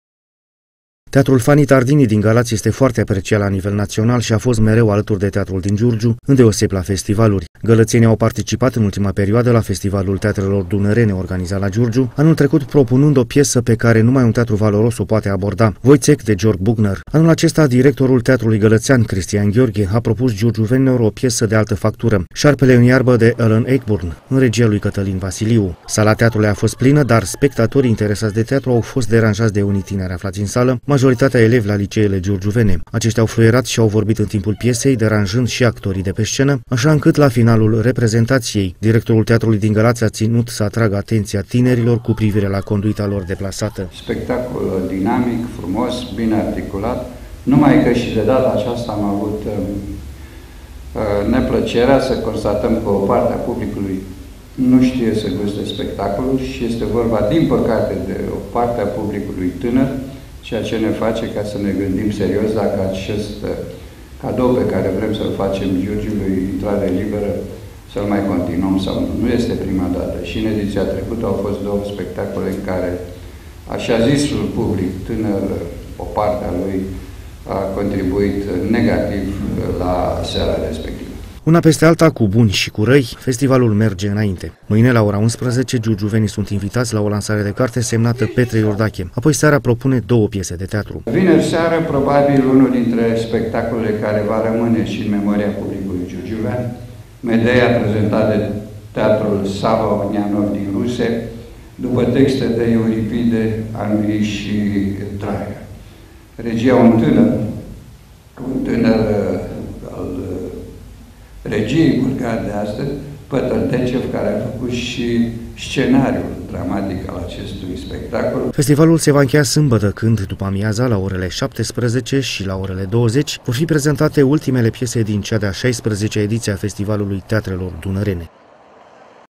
FESTIVAL - PENTRU CINE?
Proastă educație sau teribilism - așa ar putea fi caracterizată conduita unor tineri spectatori care aseară au deranjat trupa de actori de la reputatul teatru Fanni Tardini din Galați. Deși spectacolul era bine cotat, elevii prezenți în sală s-au manifestat neadecvat, deranjând ceilalți spectatori.